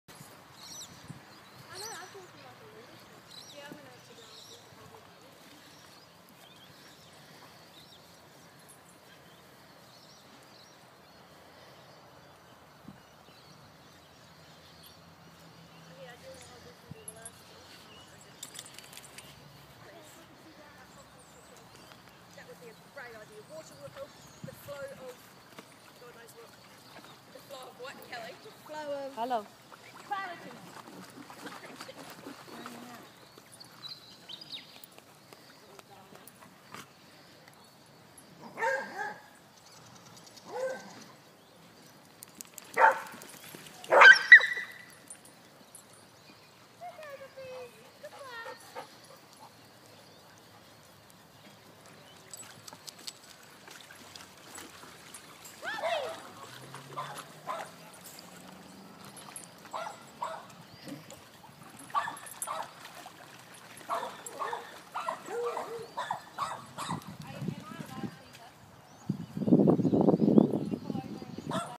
HDC 17.4.12
Boxa the beagle is infatuated with Baker the Afghan Hound.